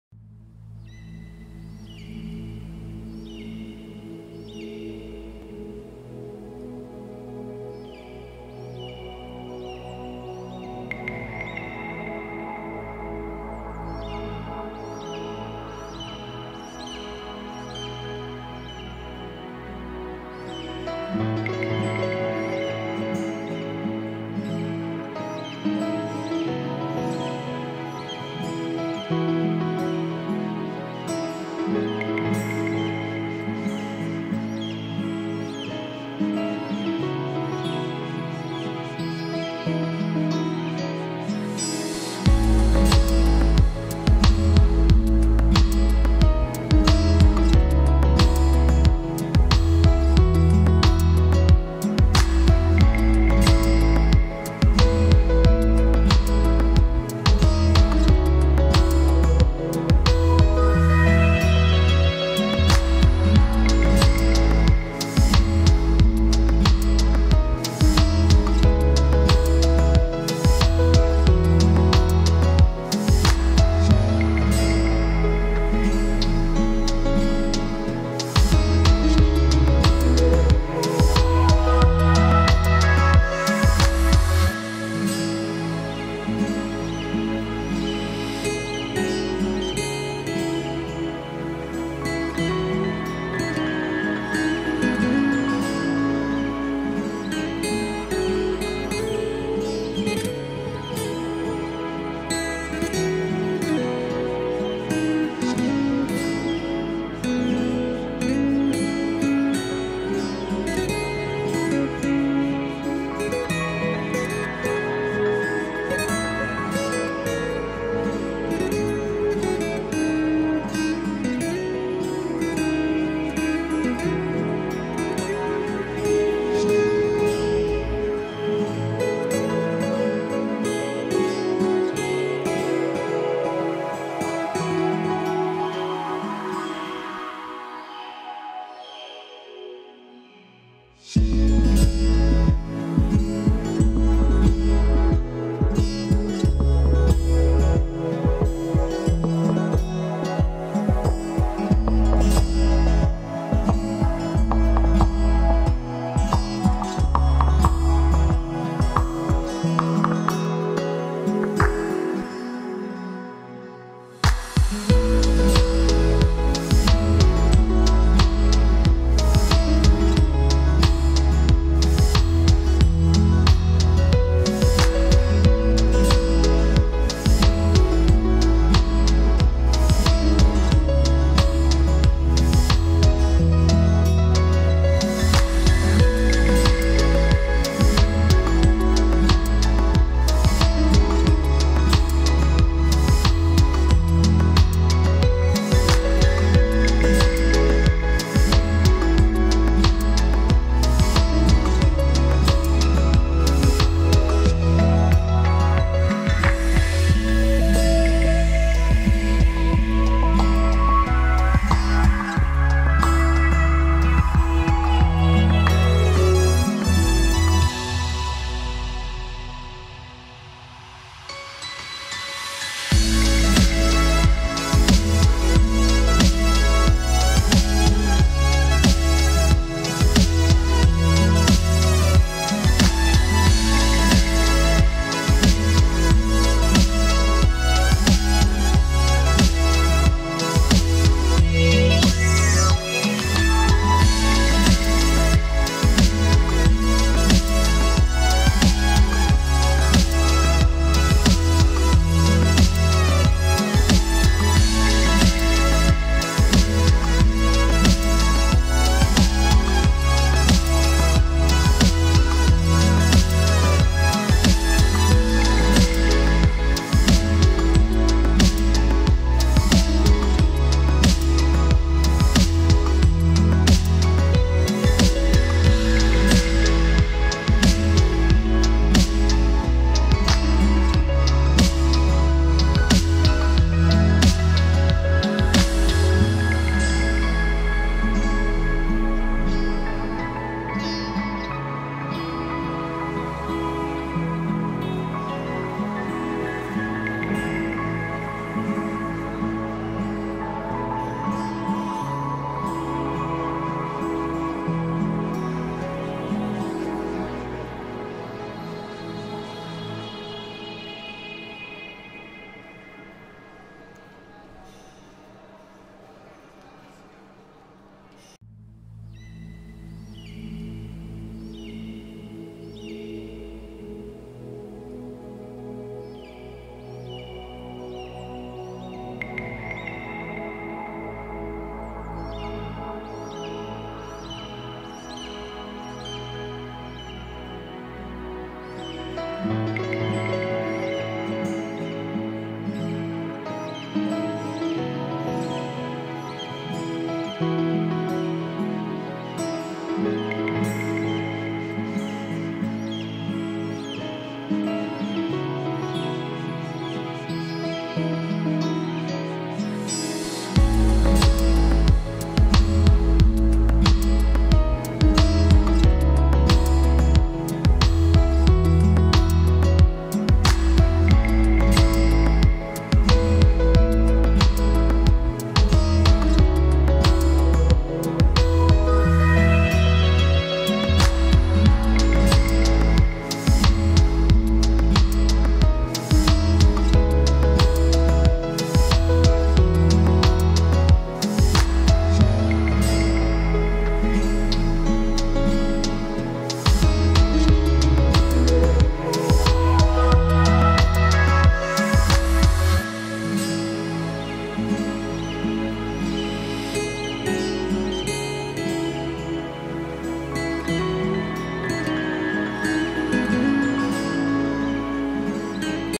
Delta 2 2012 MNBound 3/28/16 Highlights from the Day :)
Broadband Corp appreciates all of you're viewing support! MNBound.com solar powered Eagle Cam
We all want to Thank you them for this cam
Edited by EagleEyeHallock